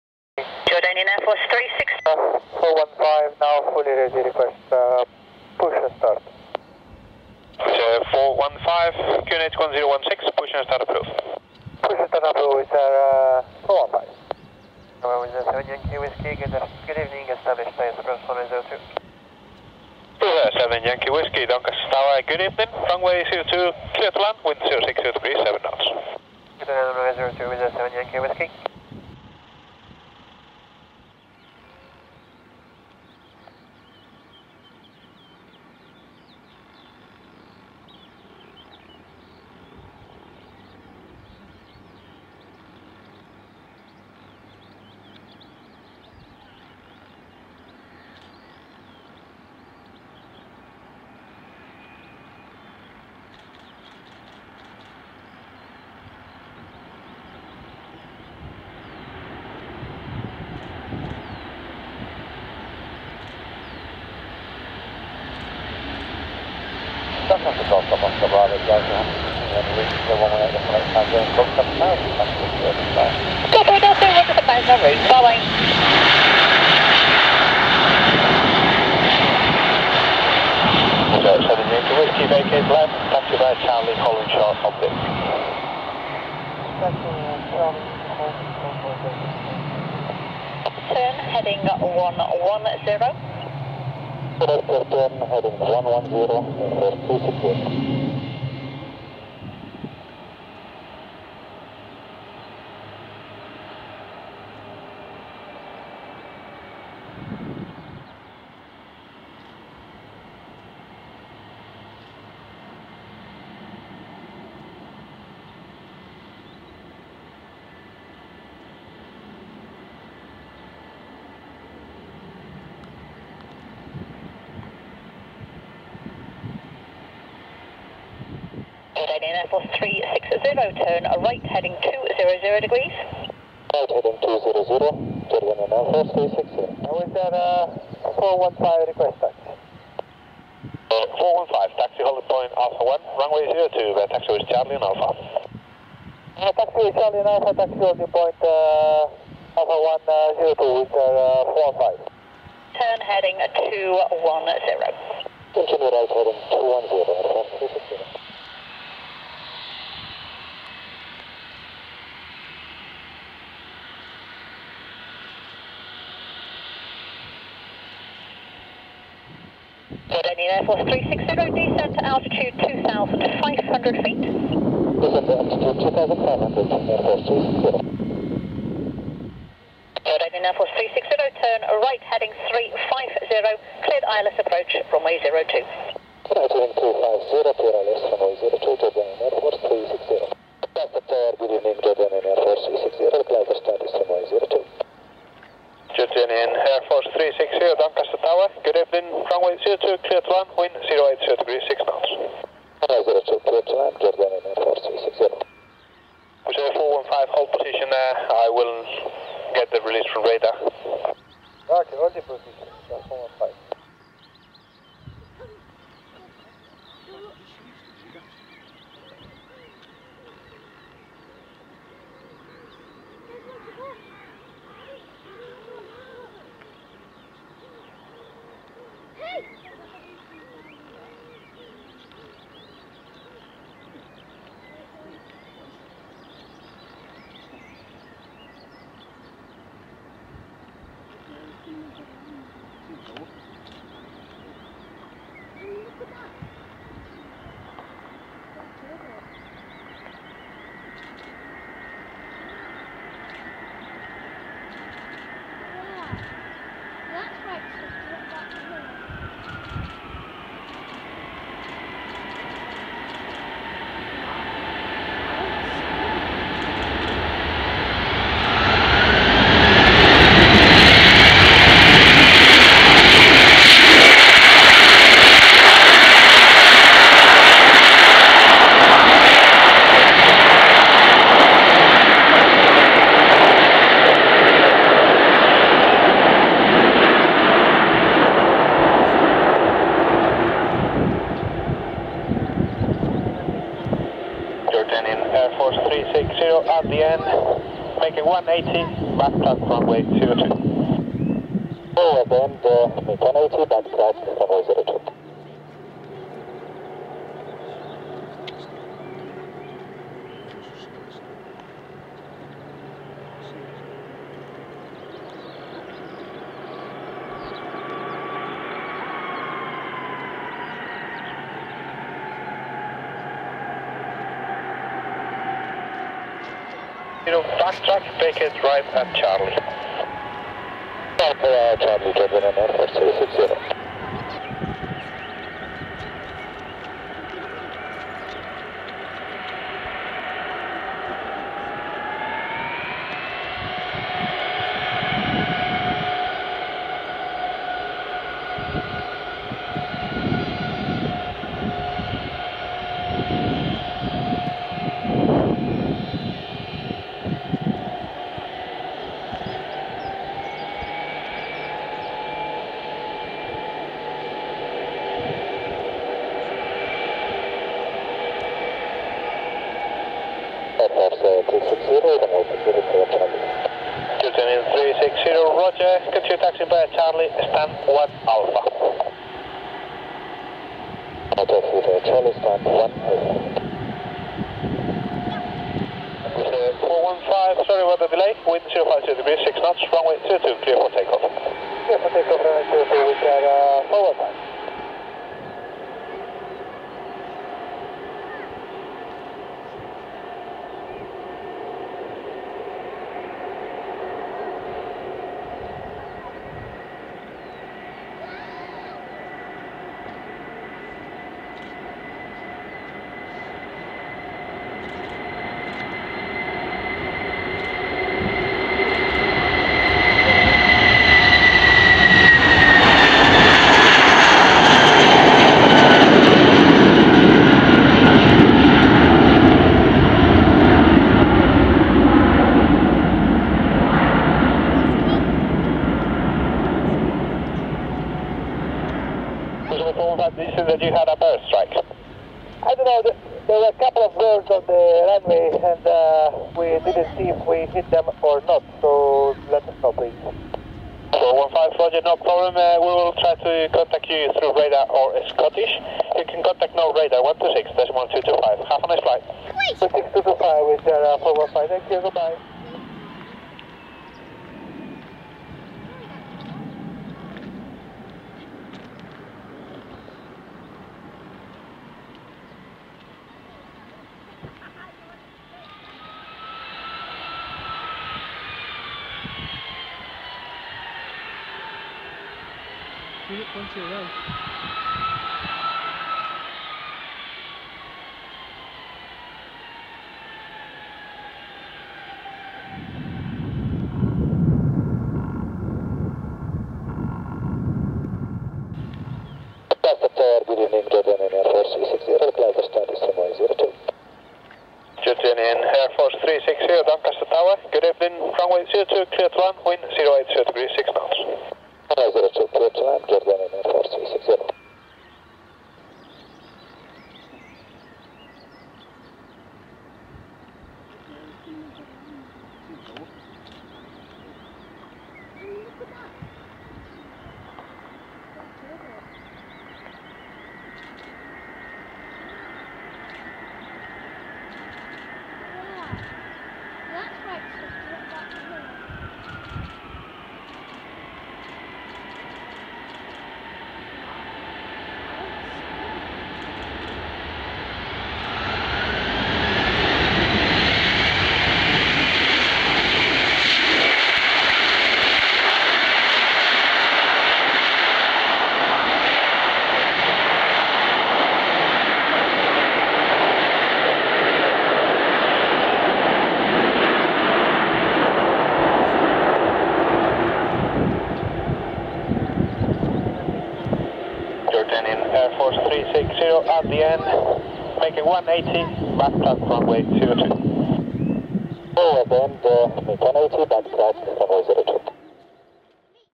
ILYUSHIN IL-76 ROYAL JORDANIAN AIRFORCE @ Doncaster Airport (UK)
No657 VIDEO OF ROYAL JORDANIAN AIR FORCE  ,
ILYUSHIN  IL-76 , 360 , RJZ360 LANDING FROM AMMAN , JORDAN .
& WIZZAIR A320 , HA-LYC , W68009 FROM VILNIUS &
HA-LWC , W62510 TO RIGA .
BY M CONNOLLY ,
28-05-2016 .